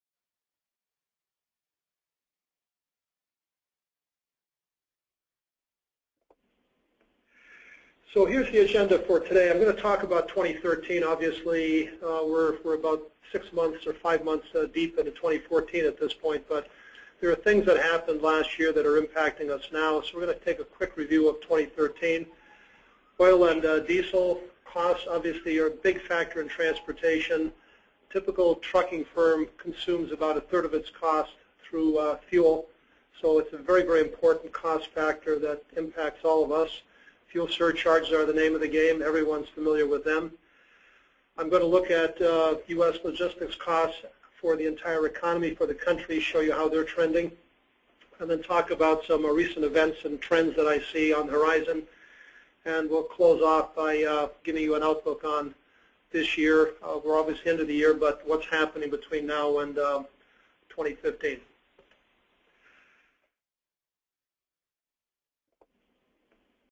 The CRITICAL Future of Logistics Webinar (Intro)
This webinar is aimed at freight broker agents dealing with carrier capacity issues.  Presenting will be world-renowned supply chain/logistics specialist, Jack Ampuja. He will discuss the constantly changing trends in the logistics industry that every freight broker needs to know to help overcome shrinking carrier capacity - deemed by many to be the single biggest obstacle freight brokers face today.